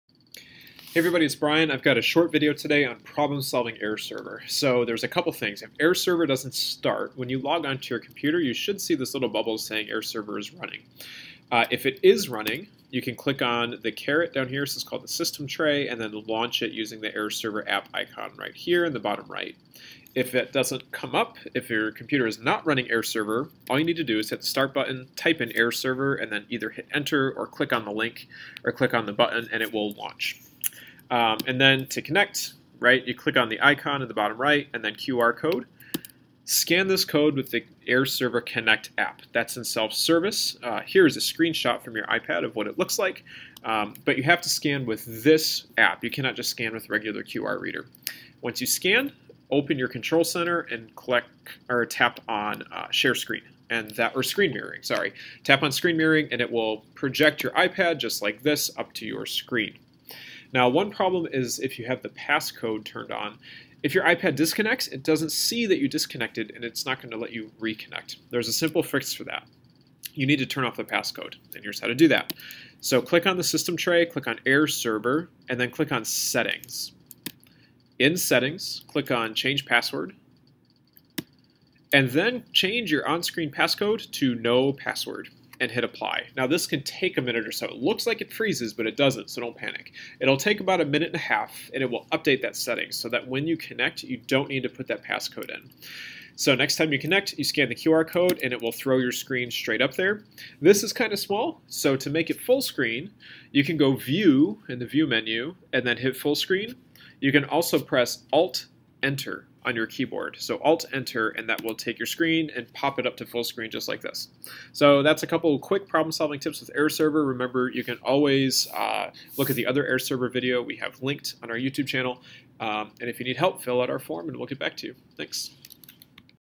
Air Server - Quick Fixes
This video has a couple of quick fixes for AirServer problems.

1. What if AirServer isn't running when I log in?
2. The QR code isn't working!
3. Clearing the password